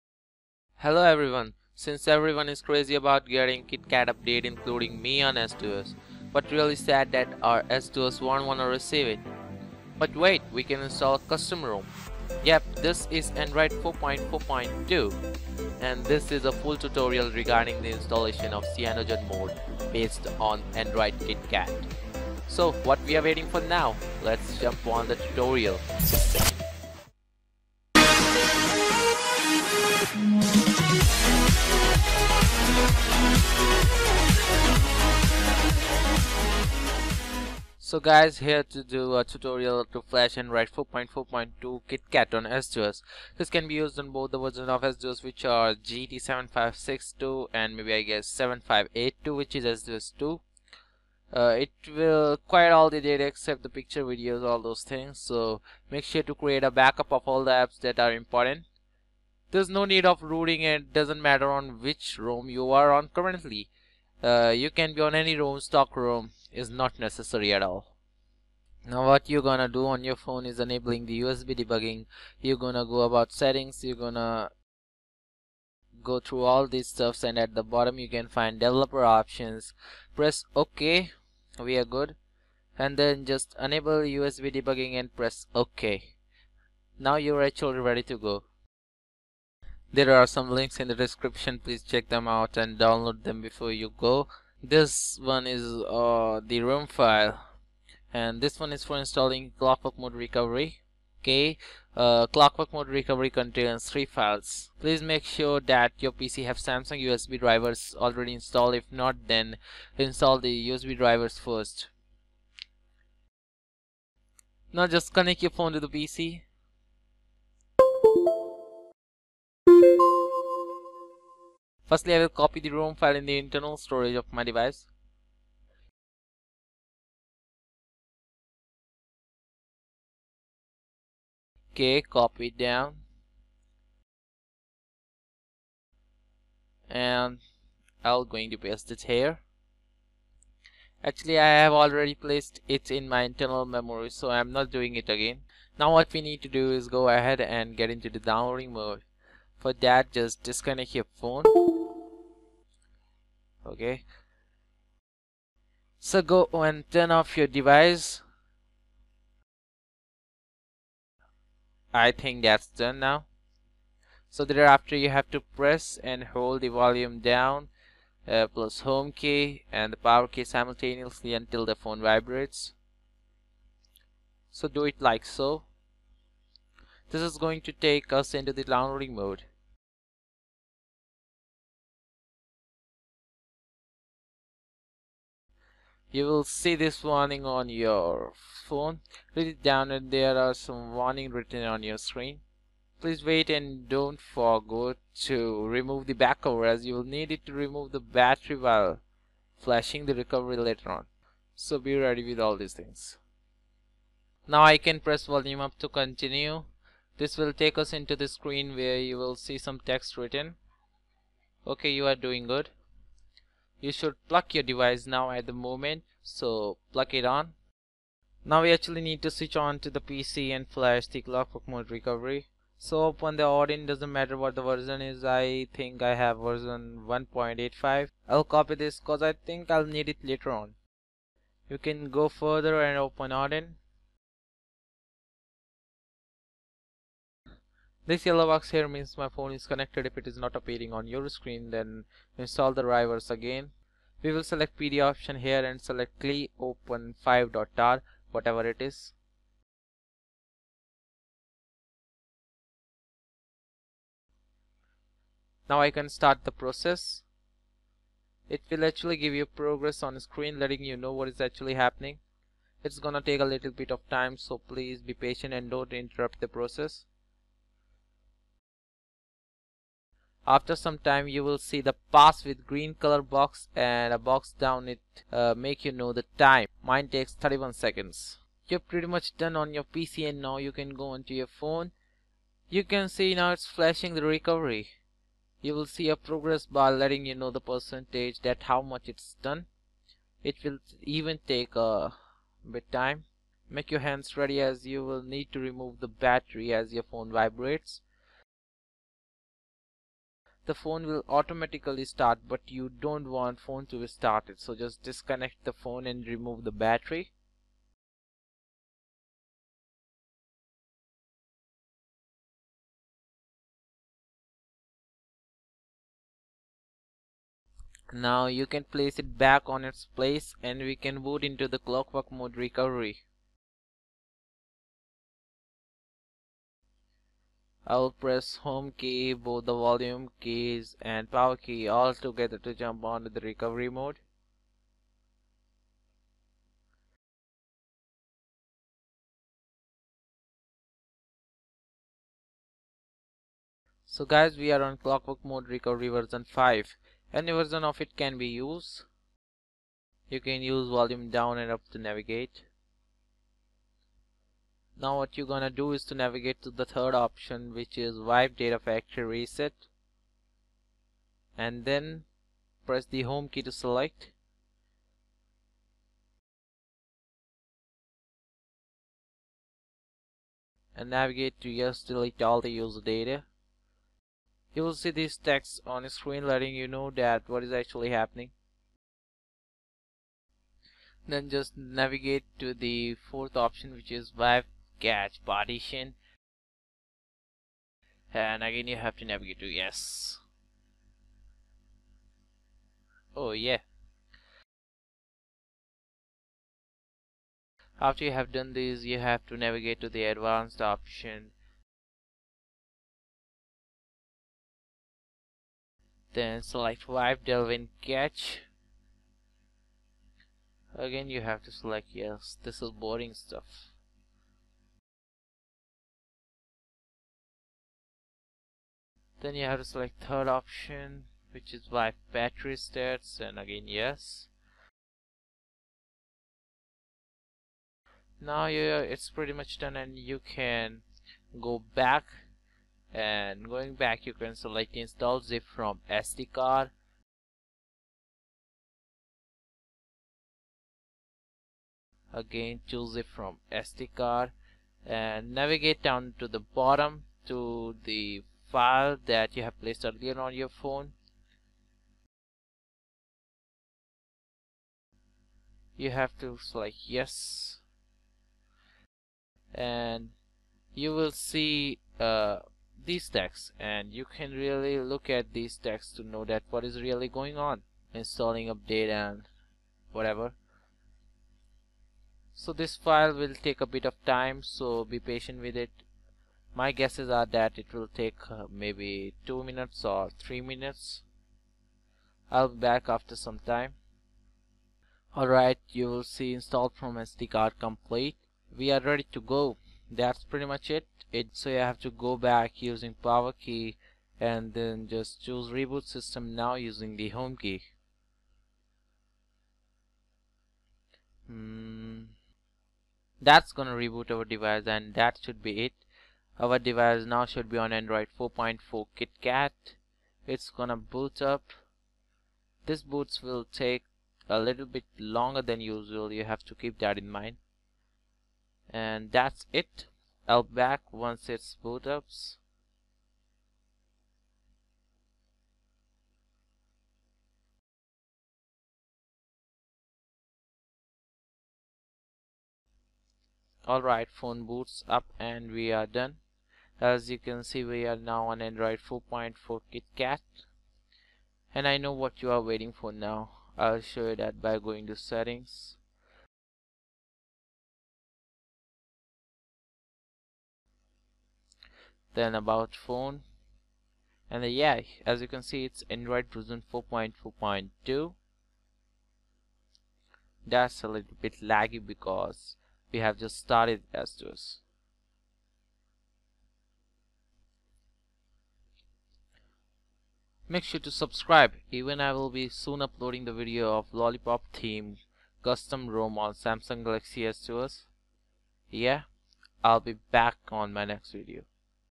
📱Install Android 4.4.2 kitkat on s duos 1 or 2 (GT-7562 Or GT-7582) (Full Tutorial)
a new video for gt 7562 has been released... with sim support...

Before going on please have a look at the non working features of rom of s duos 1...In s duos 2 rom most of the features will work...Please read the whole description carefully...
Main Files (gt 7562) (Used in video)

This rom has many lags and not working properly on s duos.
[Unofficial] CyanogenMod 11 based on android 4.4.2

What Doesn't Works:-
1)Bluetooth
2)network
3)camera upto 3mp only

A new cm 11 has been ported with network
Main files (Gt 7582)
S5 rom 4.4.2 rom for s duos 2:-